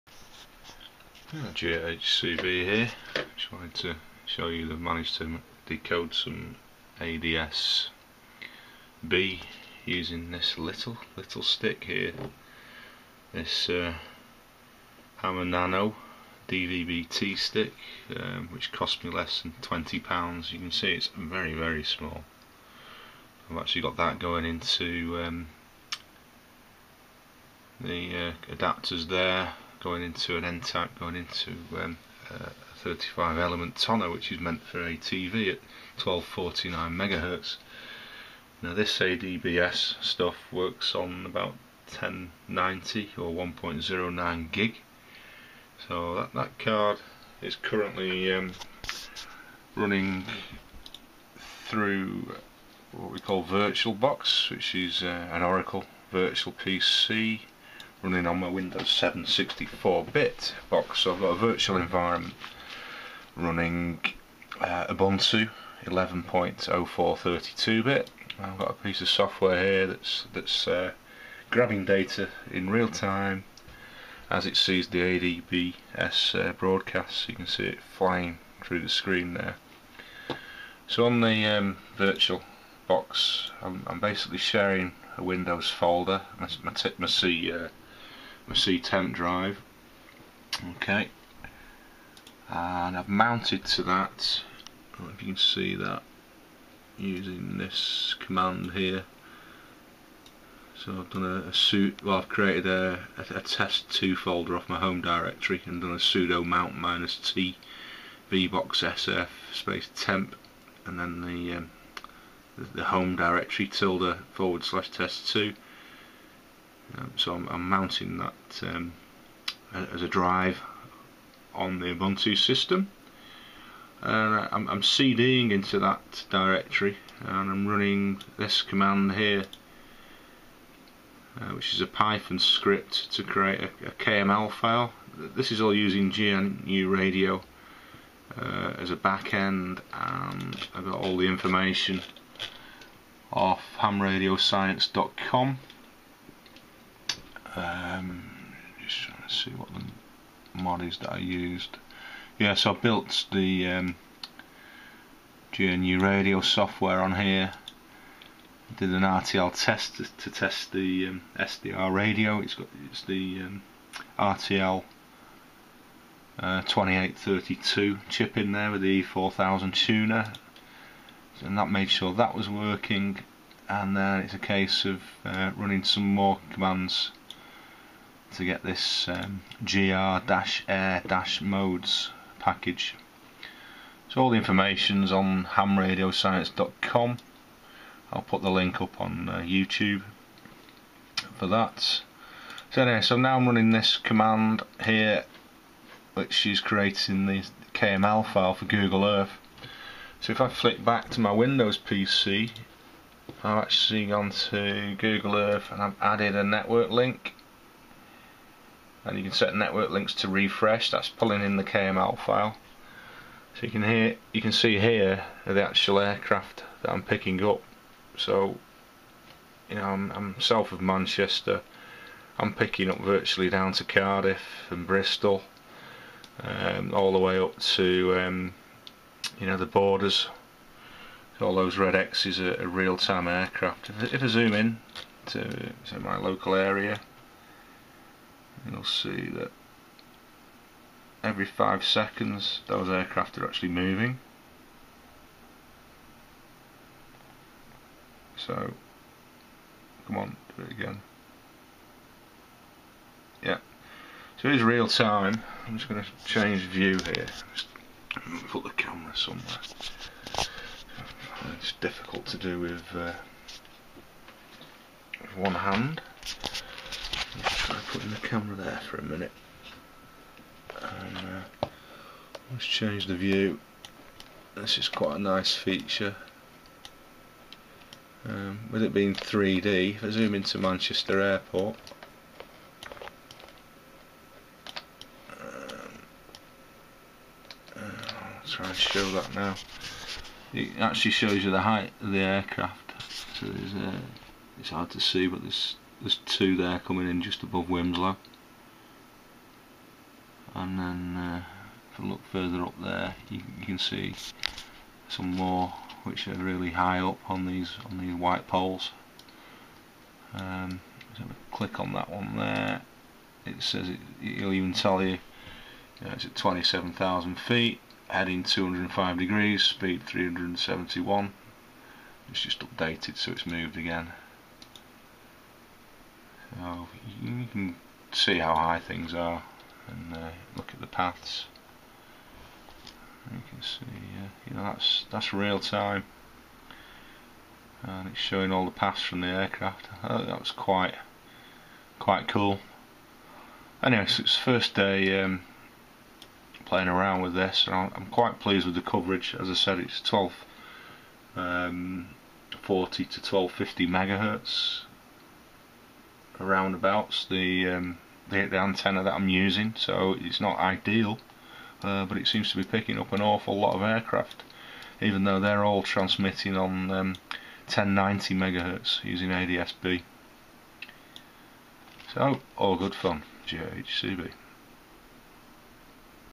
ads-b rx with a cheap rtl2832 with e4000 tuner hama nano dvb-t At the qth of G8HCB P1
Using an rtl-sdr dvb-t stick to receive ads-b broadcasts on 1090 Mhz. The rtl-sdr receiver is a hamo nano dvb-t stick running and connected under Ubuntu 11.04 which is running in virtual box on a windows 7 x64 bit host. The resulting data from the reception is piped into a kml file using a python script and monitored in realtime with 5 second refreshes in google earth.

73 de G8HCB